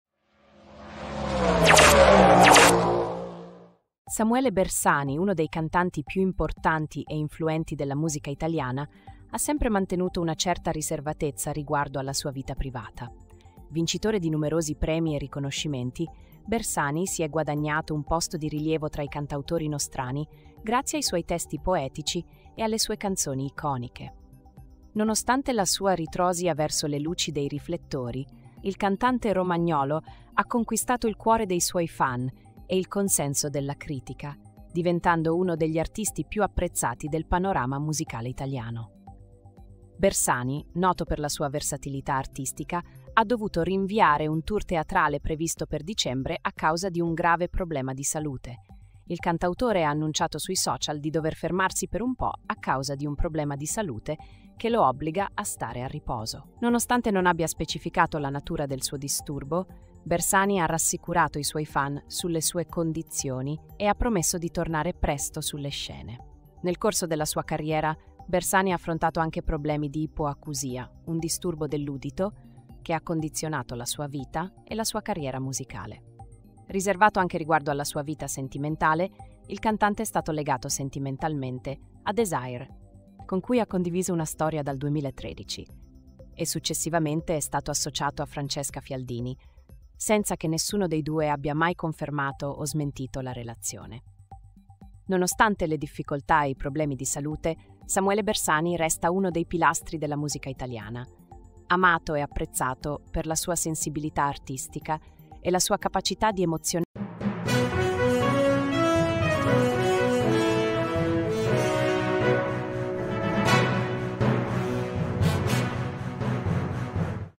SAMUELE BERSANI: GRAVE PROBLEMA DI SALUTE RINVIA IL TOUR TEATRALE
Samuele Bersani, uno dei cantanti più importanti e influenti della musica italiana, ha sempre mantenuto una certa riservatezza riguardo alla sua vita privata. Vincitore di numerosi premi e riconoscimenti, Bersani si è guadagnato un posto di rilievo tra i cantautori nostrani grazie ai suoi testi poetici e alle sue canzoni iconiche.

Nonostante la sua ritrosia verso le luci dei riflettori, il cantante romagnolo ha conquistato il cuore dei suoi fan e il consenso della critica, diventando uno degli artisti più apprezzati del panorama musicale italiano. Bersani, noto per la sua versatilità artistica, ha dovuto rinviare un tour teatrale previsto per dicembre a causa di un grave problema di salute.

Il cantautore ha annunciato sui social di dover fermarsi per un po' a causa di un problema di salute che lo obbliga a stare a riposo. Nonostante non abbia specificato la natura del suo disturbo, Bersani ha rassicurato i suoi fan sulle sue condizioni e ha promesso di tornare presto sulle scene.

Nel corso della sua carriera, Bersani ha affrontato anche problemi di ipoacusia, un disturbo dell'udito che ha condizionato la sua vita e la sua carriera musicale. Riservato anche riguardo alla sua vita sentimentale, il cantante è stato legato sentimentalmente a Desire, con cui ha condiviso una storia dal 2013, e successivamente è stato associato a Francesca Fialdini, senza che nessuno dei due abbia mai confermato o smentito la relazione.

Nonostante le difficoltà e i problemi di salute, Samuele Bersani resta uno dei pilastri della musica italiana, amato e apprezzato per la sua sensibilità artistica e la sua capacità di emozionare il pubblico con le sue canzoni.